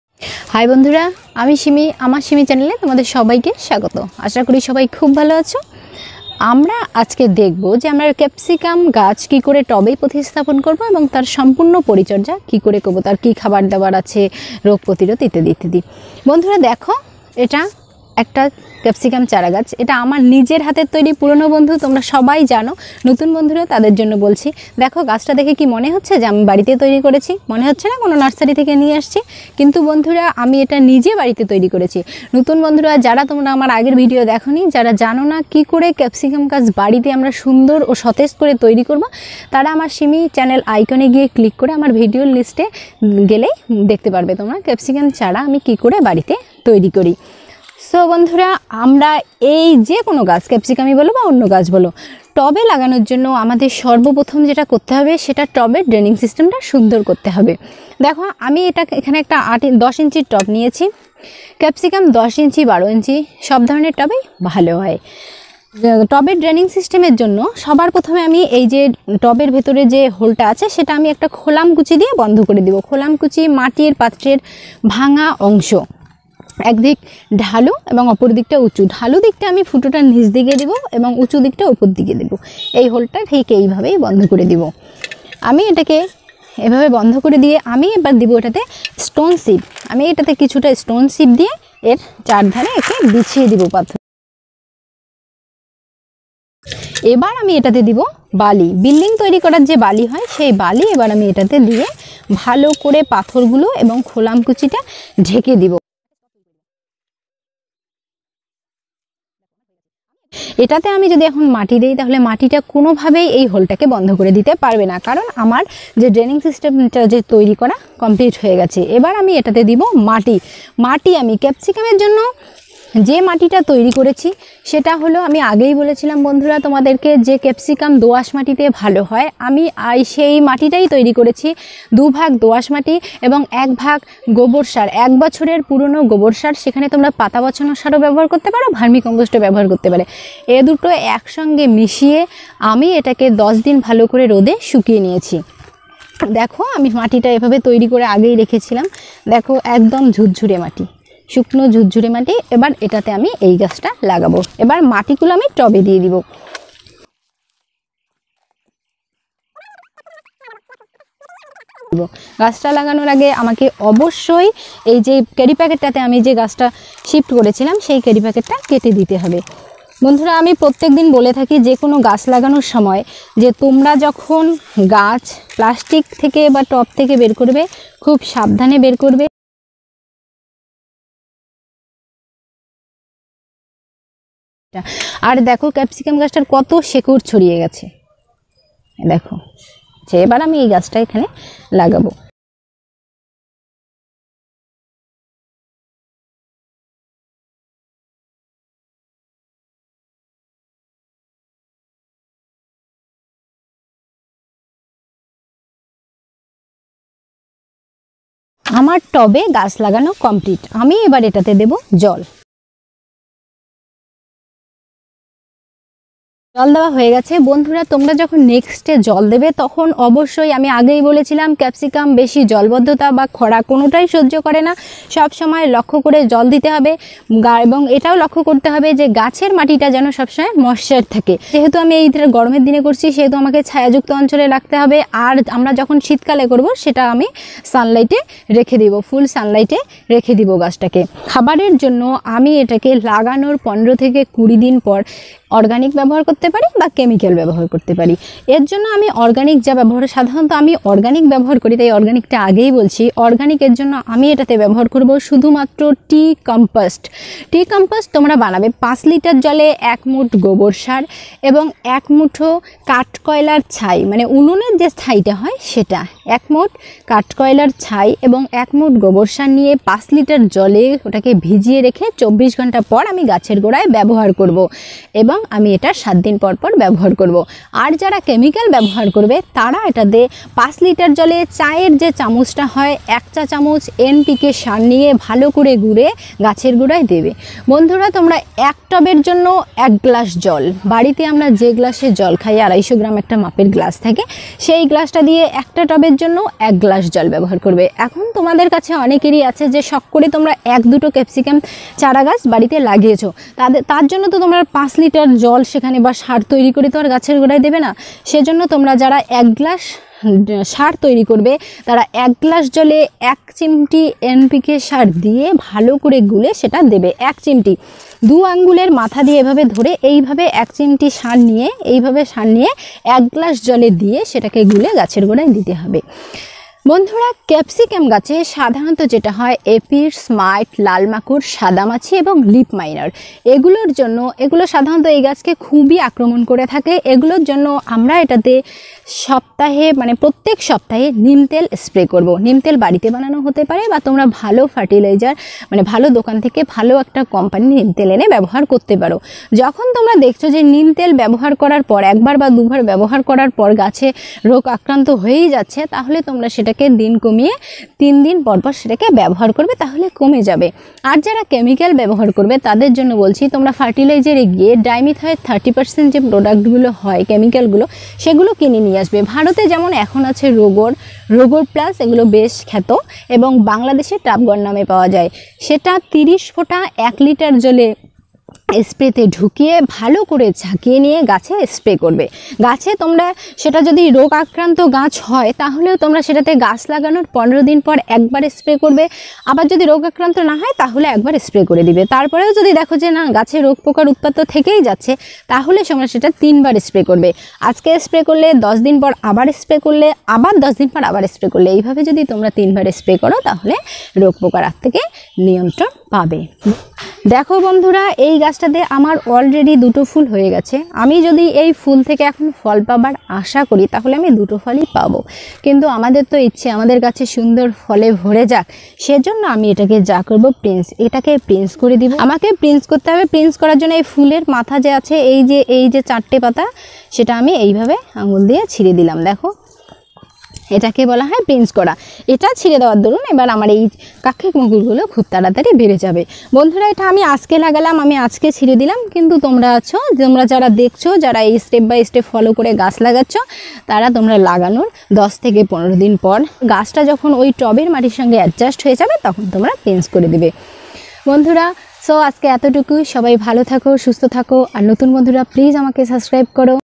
টবে কিভাবে ক্যাপসিকাম প্রতিস্থাপন, পরিচর্যা, পোকামাকড় দমন, সার প্রয়োগ How to grow capsicum in pot
বাড়িতে তৈরি করা ক্যাপসিকাম চারা গাছ কিভাবে টবে প্রতিস্থাপন করা যায়, তার পরিচর্যা ,পোকামাকড় দমন সার প্রয়োগ এবং কোথায় রাখব সেই বিষয়ে আলোচনা করা হয়েছে এই ভিডিওতে । পরবর্তী ভিডিওতে এই গাছটি থেকে কি করে সুন্দর গাছ ভর্তি ফল পাব সেই আপডেট ও দেওয়া হবে।